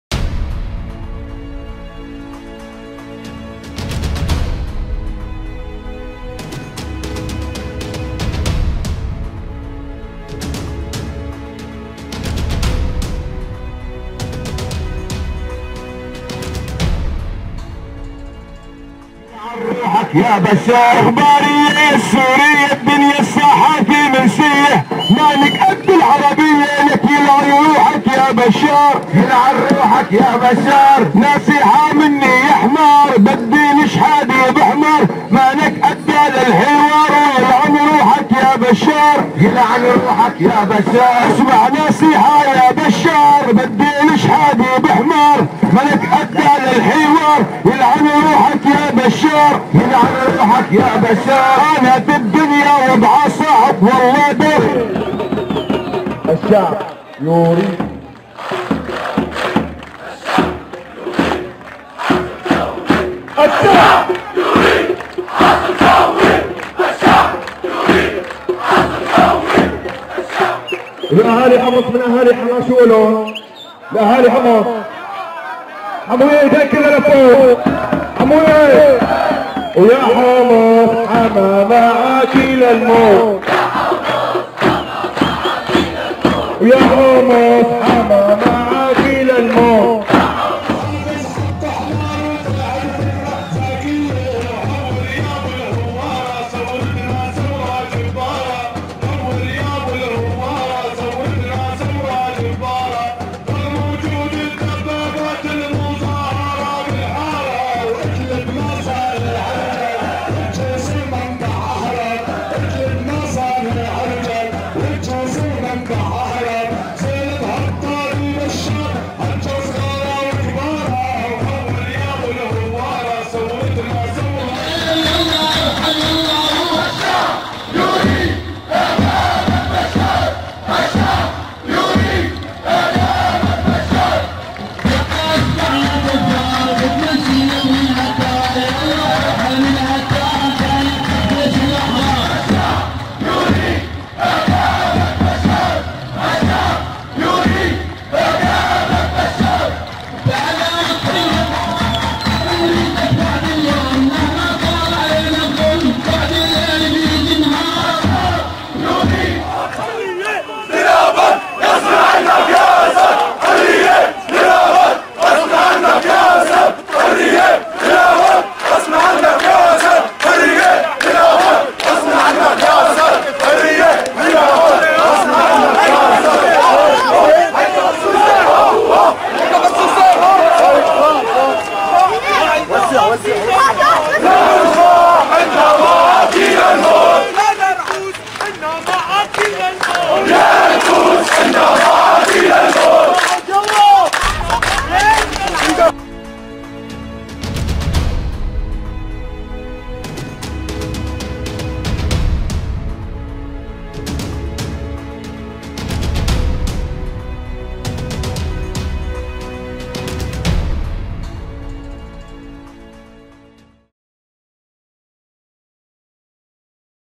من ذاكرة الثورة السورية 28/11/2015
• برنامج يوثق أهم وأبرز الأحداث التي تركت بصمتها في ثورتنا العظيمة
• ومن أهمها تلك التي حدثت في مثل هذا اليوم